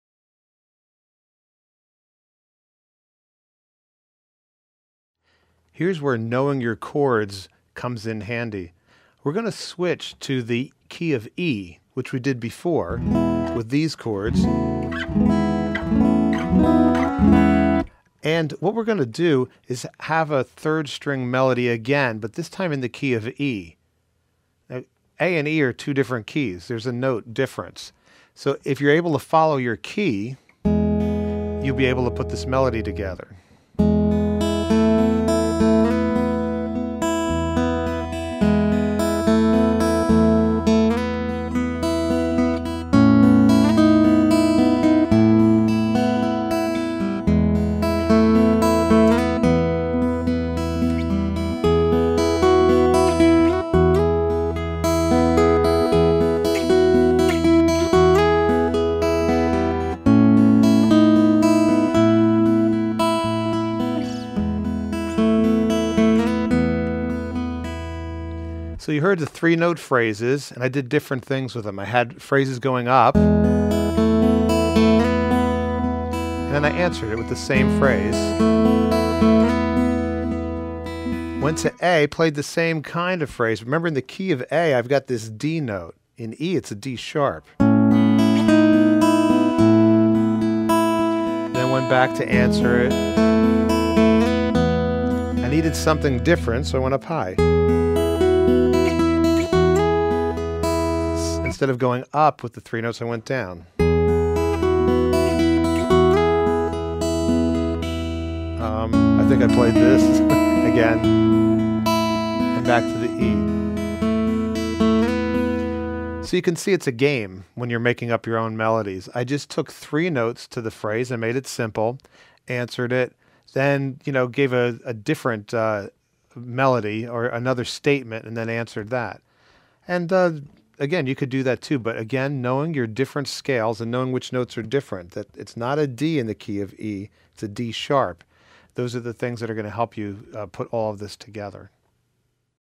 Acoustic Guitar Lessons - Fretboard Epiphanies - Melody in E
Here is the same idea in the Key of E. You will go through some trials and errors as you learn the craft of writing a melody. After you spend some time at this, you will probably learn to improvise some great lines. Notice the phrasing again. I'll use short, sometimes three note phrases, to make a musical statement. Then, I'll answer it with a parallel phrase or a contrary phrase. If you analyze the structure of the melody, you will find the patterns.